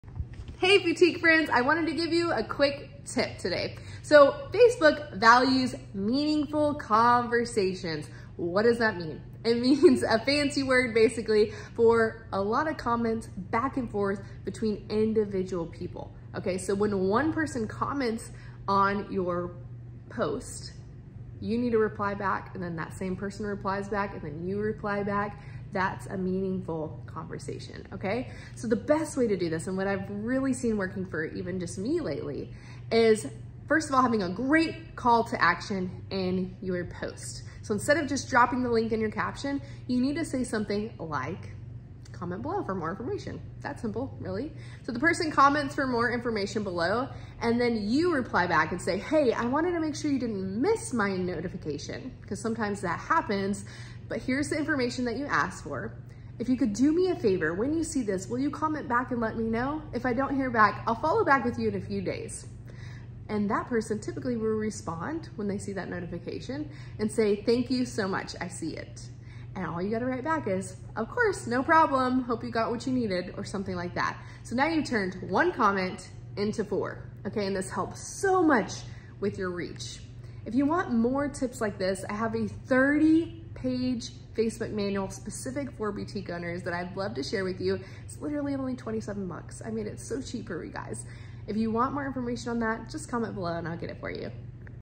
Revive a dead Facebook group with these boutique owner tips
Are you a boutique owner with a dead Facebook group, watch this!